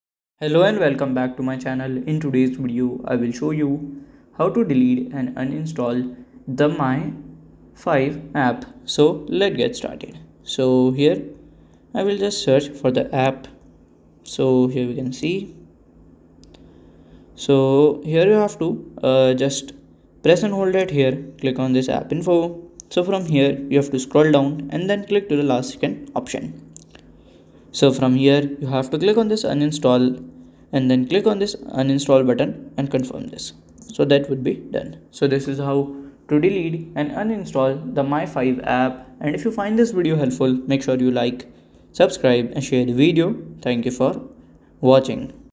✅ How To Uninstall/Delete/Remove My5 App (Full Guide)
In this comprehensive guide, we provide you with a detailed walkthrough on how to uninstall, delete, or remove the My5 app from your device. Whether you're looking to free up storage space or you've decided to stop using the app for streaming your favorite TV shows, we've got you covered. Follow our step-by-step instructions to ensure a smooth removal process and bid farewell to the My5 app hassle-free.

Key Learning Points:
- Step-by-step guide to uninstalling the My5 app
- Deleting the app securely from your device